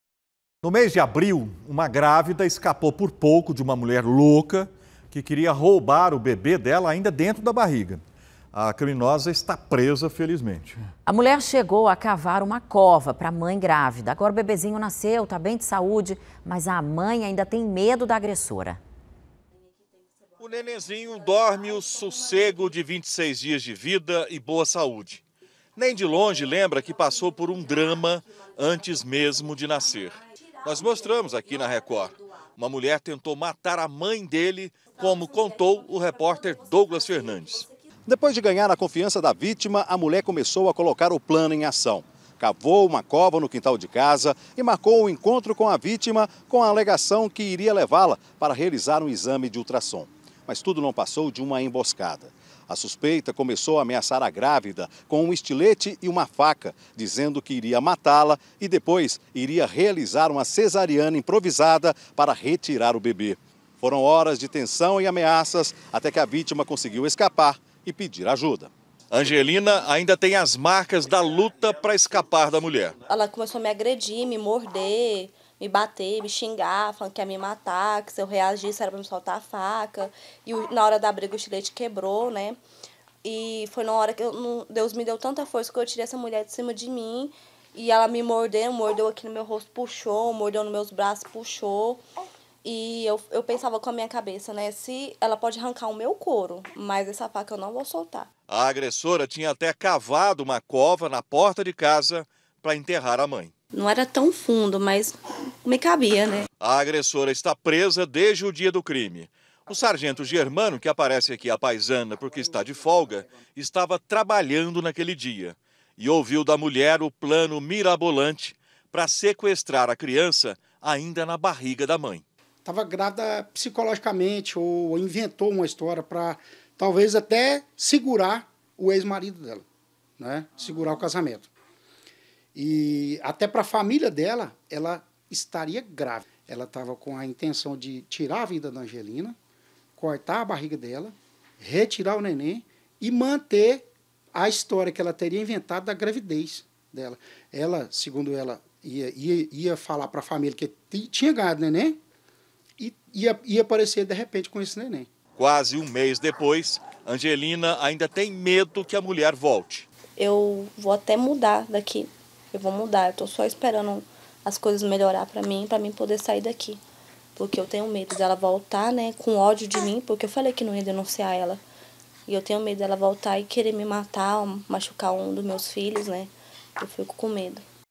JUNTO DA MÃE: BEBÊ QUASE ROUBADO AINDA NA BARRIGA ESTÁ BEM
NO MÊS DE ABRIL UMA GRÁVIDA ESCAPOU POR POUCO DE UMA MULHER QUE QUERIA ROUBAR O BEBÊ DELA AINDA DENTRO DA BARRIGA DELA. A CRIMINOSA ESTÁ PRESA.